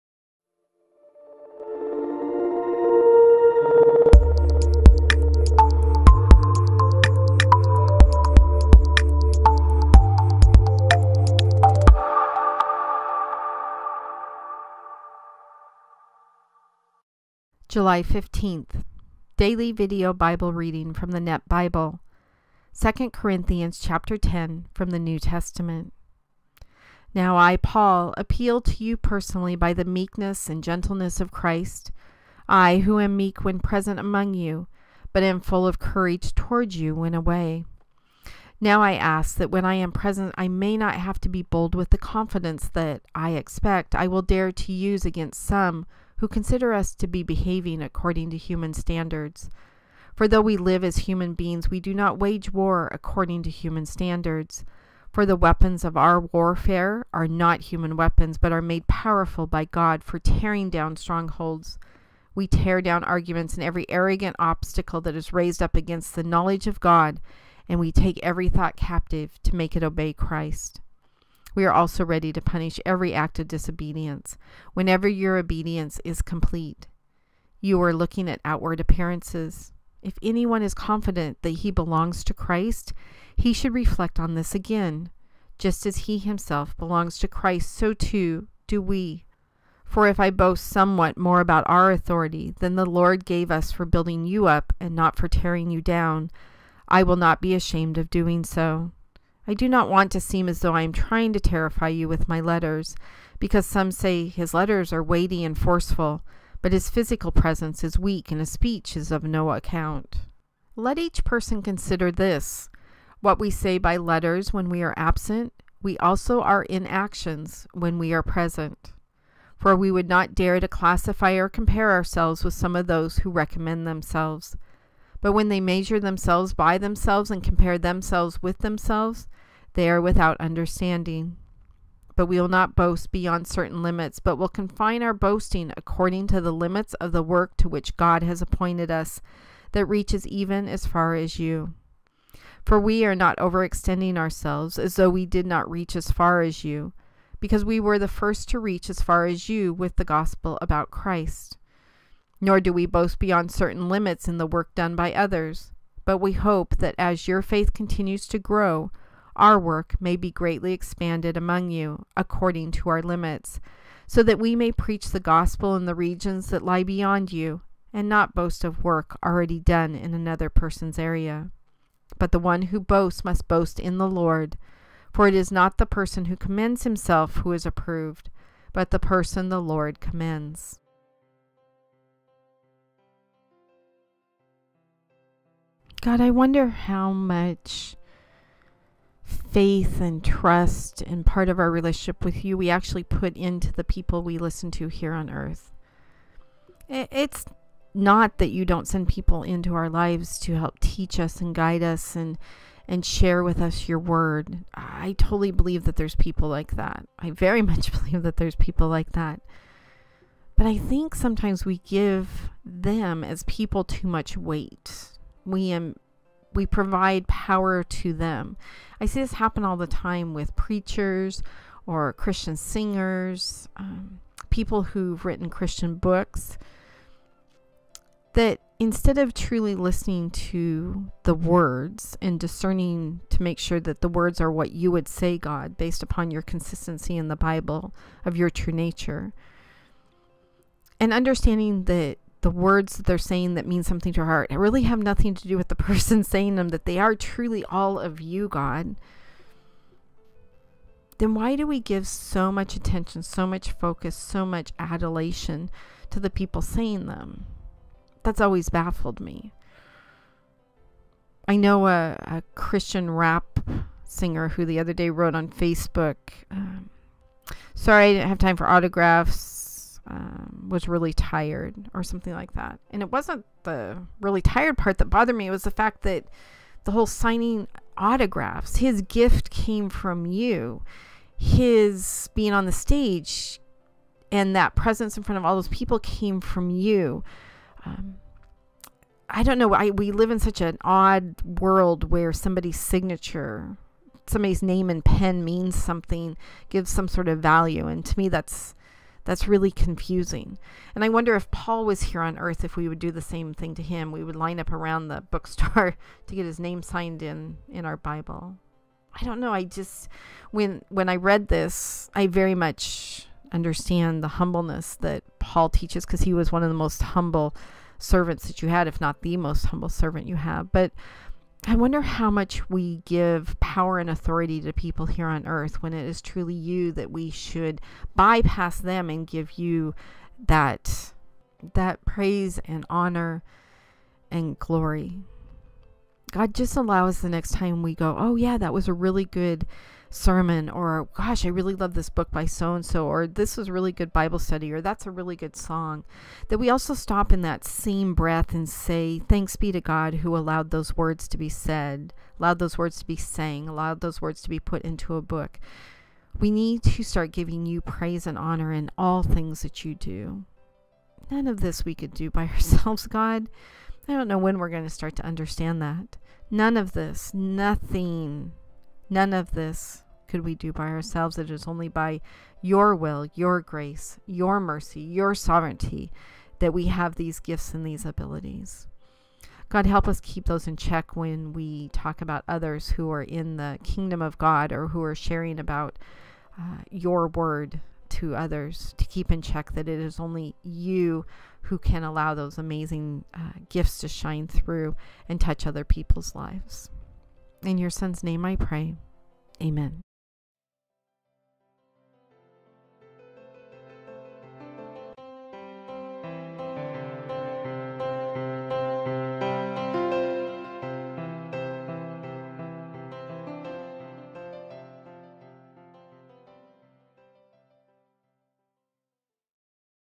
July 15 2 Corinthians 10 New Testament for the Daily Video Bible DVB
2 Corinthians 10:1-18 - Paul's Authority from the Lord
 10:1 Now I, Paul, appeal to you personally by the meekness and gentleness of Christ (I who am meek when present among you, but am full of courage toward you when away!) -- 10:2 now I ask that when I am present I may not have to be bold with the confidence that (I expect) I will dare to use against some who consider us to be behaving according to human standards. 10:3 For though we live as human beings, we do not wage war according to human standards, 10:4 for the weapons of our warfare are not human weapons, but are made powerful by God for tearing down strongholds. We tear down arguments 10:5 and every arrogant obstacle that is raised up against the knowledge of God, and we take every thought captive to make it obey Christ. 10:6 We are also ready to punish every act of disobedience, whenever your obedience is complete. 10:7 You are looking at outward appearances. If anyone is confident that he belongs to Christ, he should reflect on this again: Just as he himself belongs to Christ, so too do we. 10:8 For if I boast somewhat more about our authority that the Lord gave us for building you up and not for tearing you down, I will not be ashamed of doing so. 10:9 I do not want to seem as though I am trying to terrify you with my letters, 10:10 because some say, "His letters are weighty and forceful, but his physical presence is weak and his speech is of no account." 10:11 Let such a person consider this: What we say by letters when we are absent, we also are in actions when we are present.
Paul's Mission
 10:12 For we would not dare to classify or compare ourselves with some of those who recommend themselves. But when they measure themselves by themselves and compare themselves with themselves, they are without understanding. 10:13 But we will not boast beyond certain limits, but will confine our boasting according to the limits of the work to which God has appointed us, that reaches even as far as you. 10:14 For we were not overextending ourselves, as though we did not reach as far as you, because we were the first to reach as far as you with the gospel about Christ. 10:15 Nor do we boast beyond certain limits in the work done by others, but we hope that as your faith continues to grow, our work may be greatly expanded among you according to our limits, 10:16 so that we may preach the gospel in the regions that lie beyond you, and not boast of work already done in another person's area. 10:17 But the one who boasts must boast in the Lord. 10:18 For it is not the person who commends himself who is approved, but the person the Lord commends.